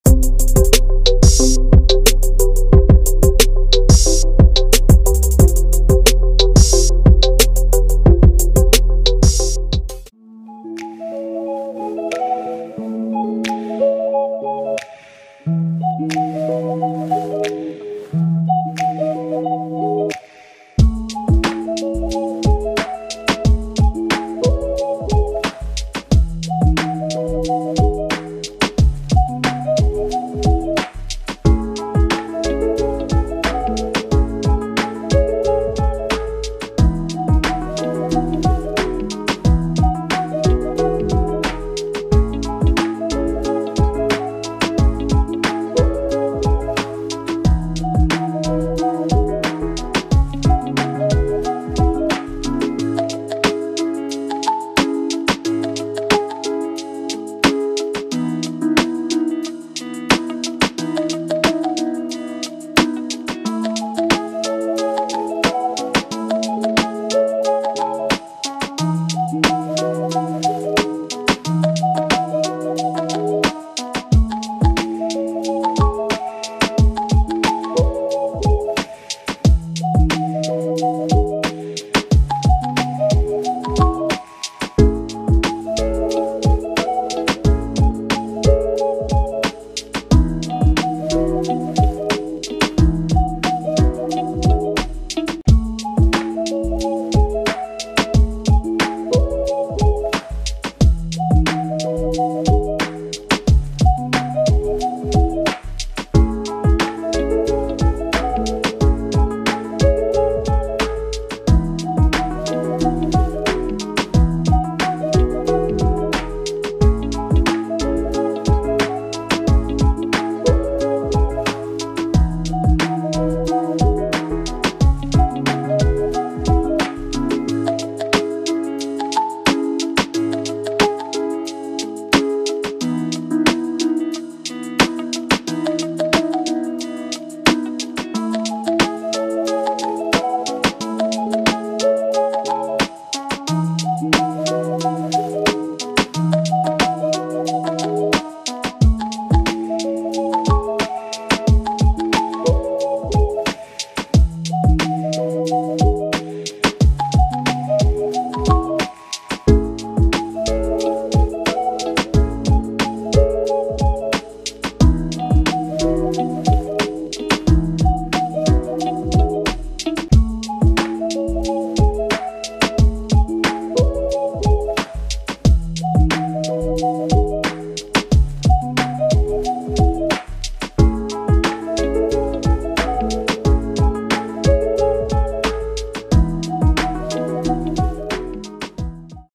Aradonga Picnic Spot || Family time || Meghalaya|| @Ghy_boyz@Lets__Explore
Cold and clear water flows through this beautiful water way and a marvelling hanging bridge above the flowing river...Spent a quality time with the family of Christ❤️